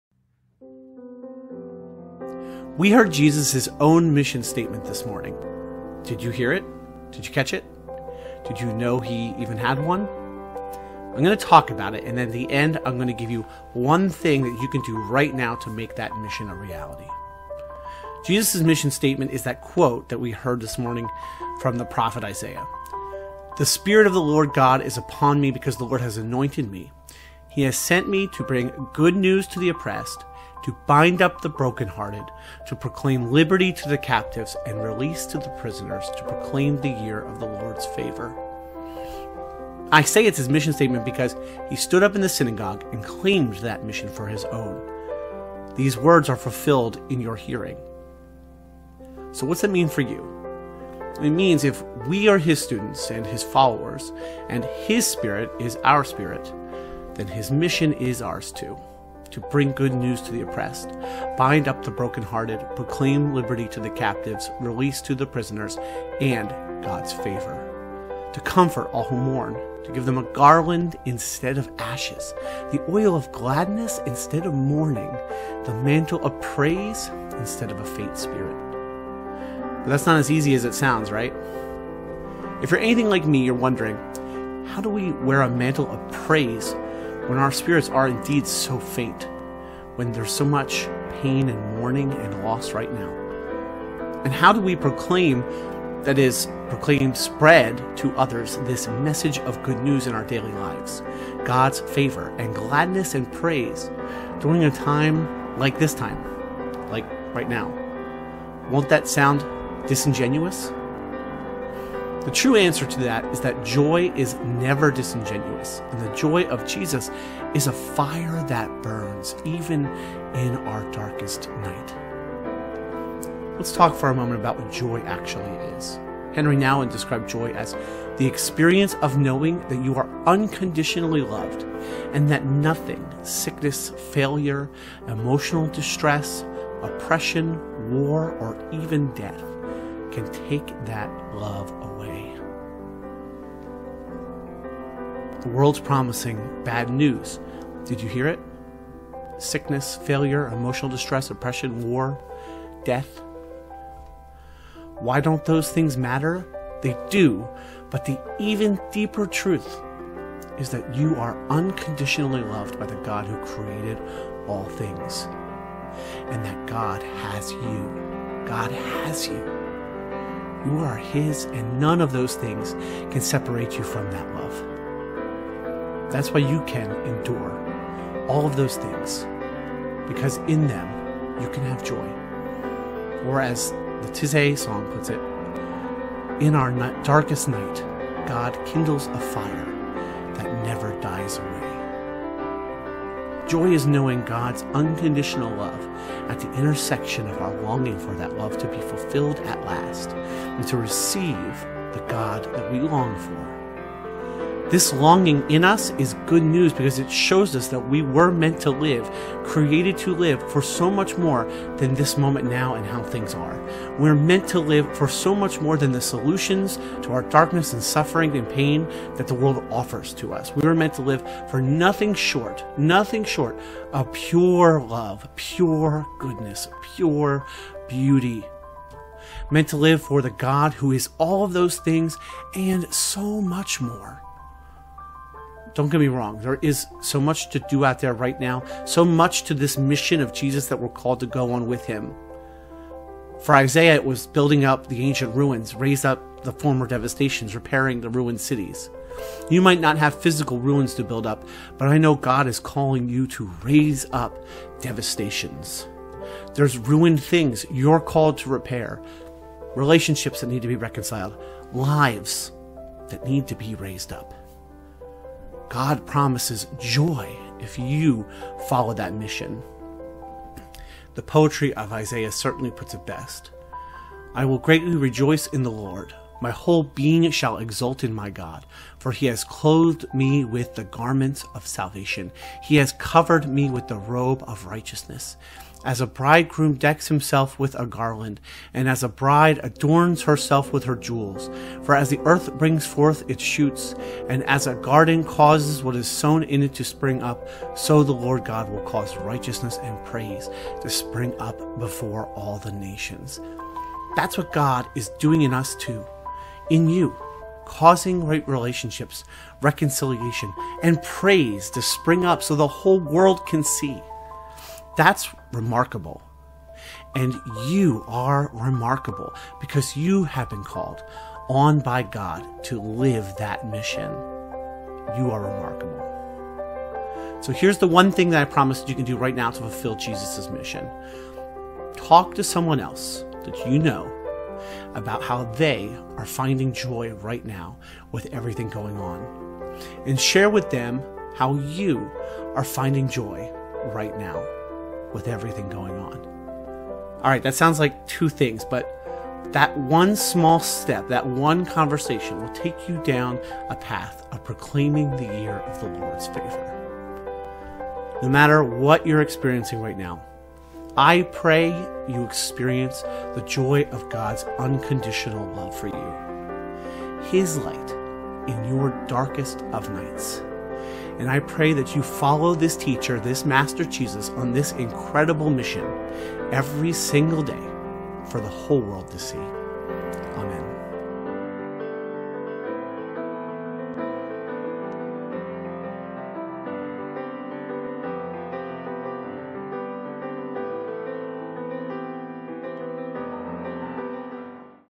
What is Joy? a sermon for Advent
What is joy?

Henri Nouwen described joy as "the experience of knowing that you are unconditionally loved and that nothing – sickness, failure, emotional distress, oppression, war, or even death - can take that love away."

No matter what you're going through right now this video will give you 1 thing you can do to spread joy and God's mission today.

Sermon for Incarnation Holy Sacrament Episcopal Church for Sunday December 13, 2020 the Third Sunday in Advent.